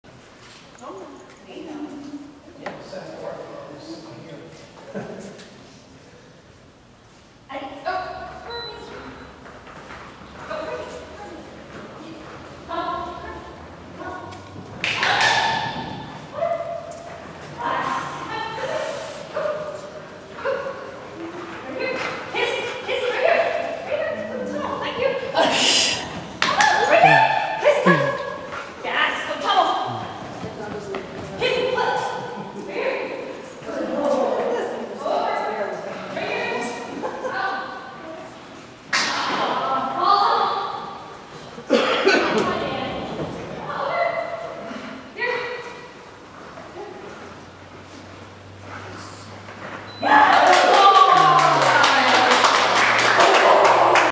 UKI Agility Trial 052116 Speedstakes Novice
Our speedstakes Novice run..  unfortunately we had a dropped bar.. and again some messy handling by me..